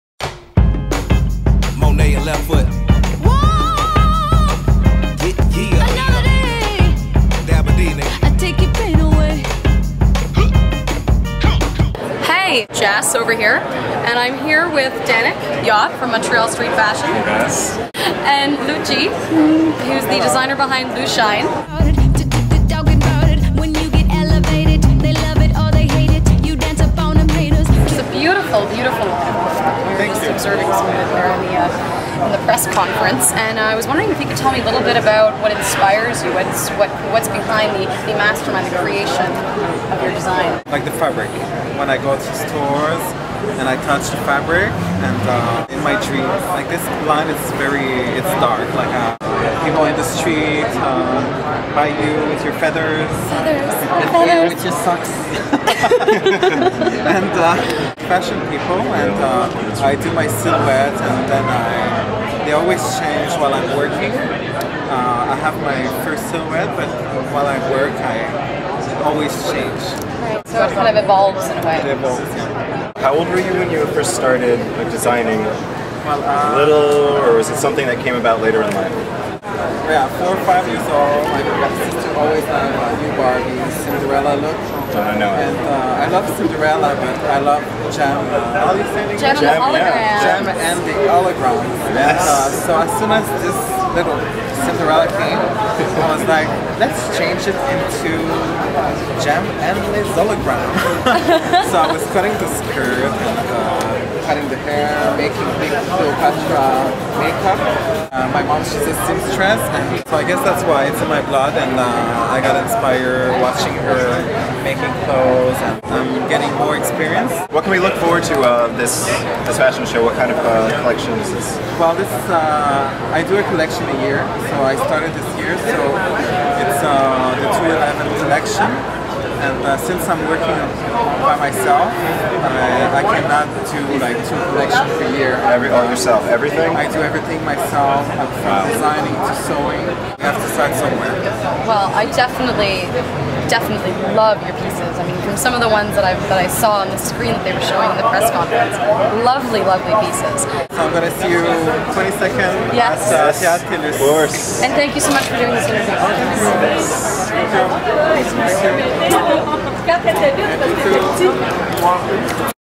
FashionBites: LuShyne
Nuda and Danik interview Lucci, designer and owner of LuShyne, a local Montreal clothing line.